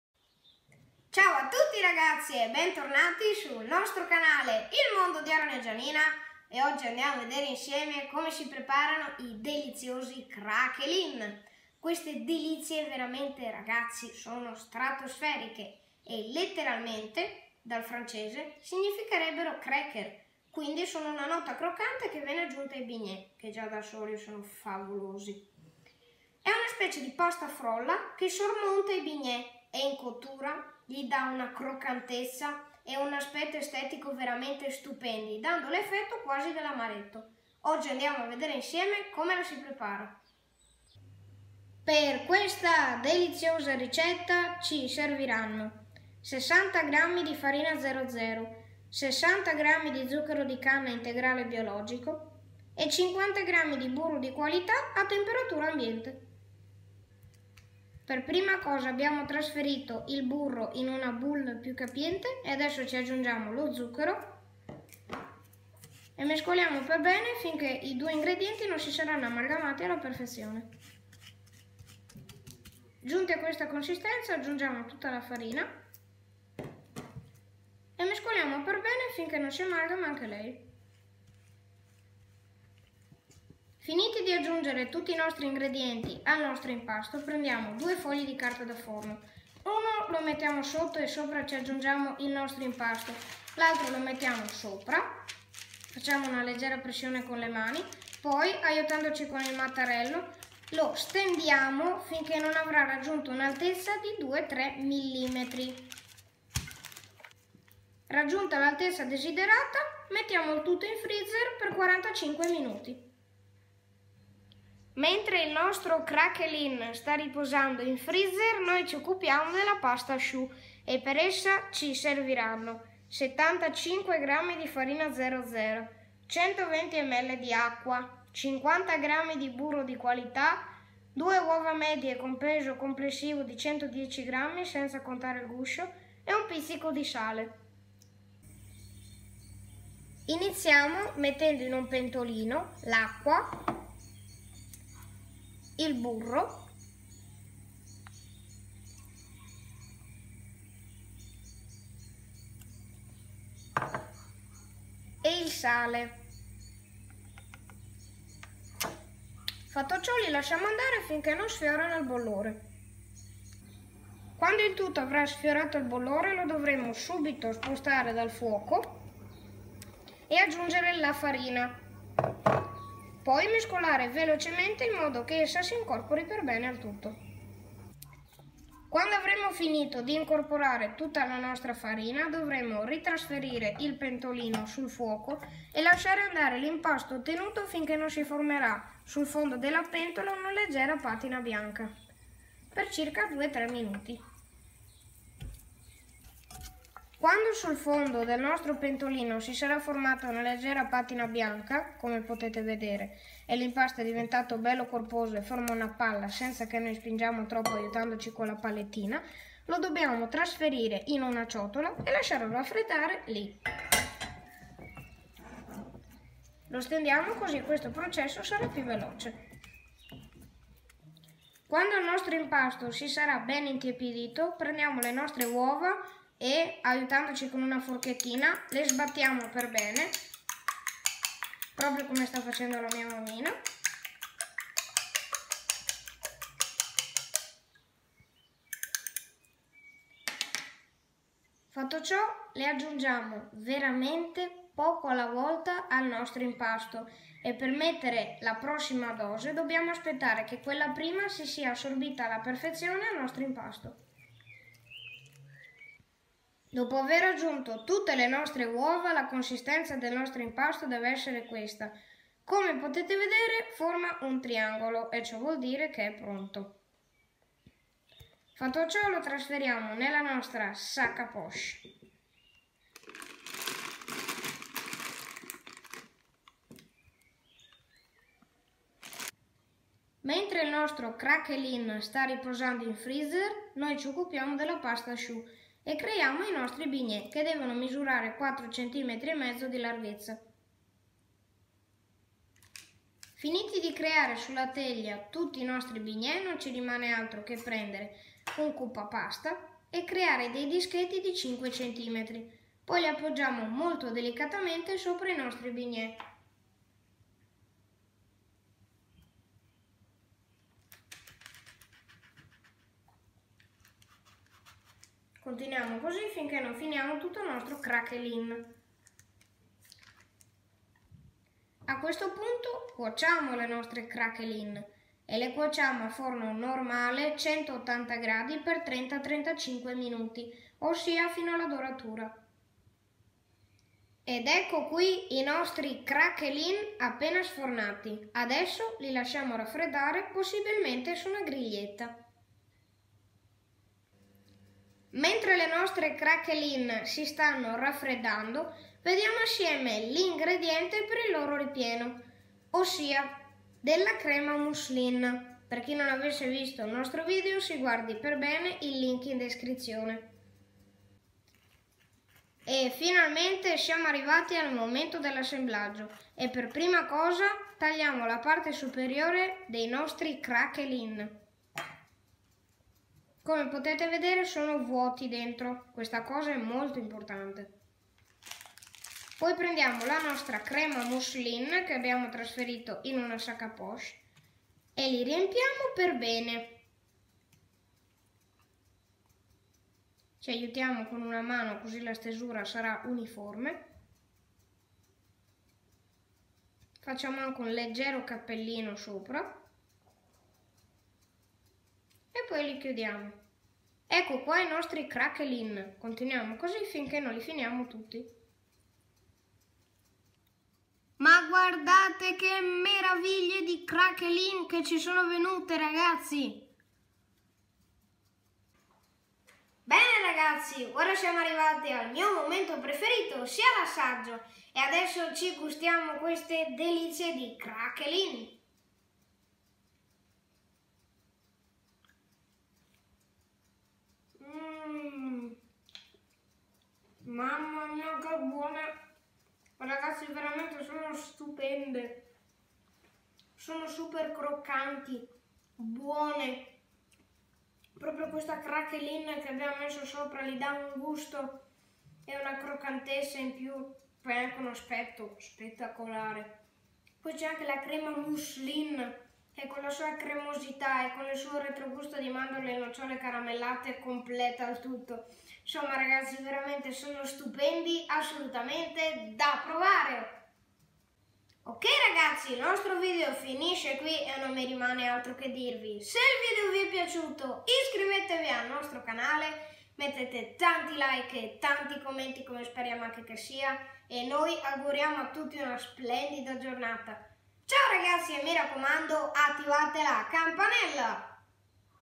Choux au Craquelin /Cream Mousseline
Ciao carissimi, oggi abbiamo il grande piacere di presentarvi una nuova ricetta, molto semplice e alla portata di tutti, quale vi darà tantissime soddisfazioni in famiglia: I BIGNÉ CRAQUELIN, che non sono altro che dei bignè avvolti da una "cupola" di una frolla particolare, che in cottura avvolge tutto il bignè e fa si che, appena sfornato, abbia quella particolare croccantezza e quello stupendo aspetto estetico di cui è divenuto famoso. Il ripieno, ossi la nostra crema mousseline, completa questo dolce in un'esplosione di sapori.

Per questa deliziosa ricetta ci serviranno:
PER IL CRAQUELIN:
60g di farina "00"
60g di zucchero di canna integrale BIO
50g di burro a temperatura ambiente di alta qualità

PER LA PASTA CHOUX:
75g di farina "00"
120ml di acqua
50ml di burro
2 uova con peso complessivo di 110g, senza contare il guscio
1 pizzico di sale

PER IL RIPIENO:
La nostra crema mousseline

🇺🇸 For this delicious recipe we will need: 🇺🇸
FOR THE CRAQUELIN:
60g of "00" flour
60g of whole meal BIO cane sugar
50g of butter at high quality room temperature

FOR PASTA CHOUX:
75g of "00" flour
120ml of water
50g of butter
2 eggs with a total weight of 110g, without counting the shell
1 pinch of salt

FOR THE STUFFING:
Our mousseline cream